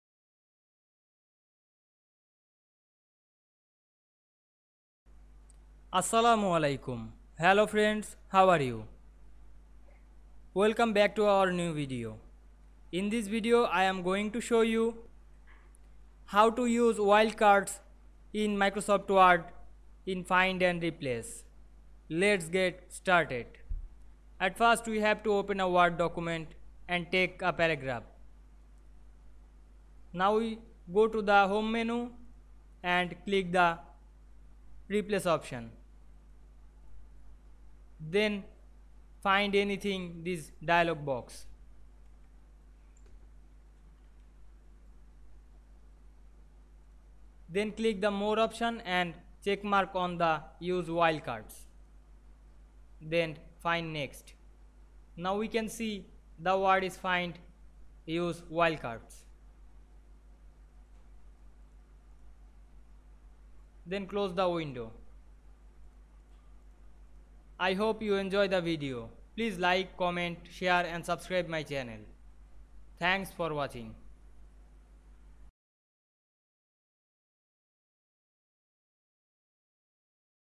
How to use wildcards in Microsoft word find and replace
In this video I will show you: How to use wildcards in Microsoft word find and replace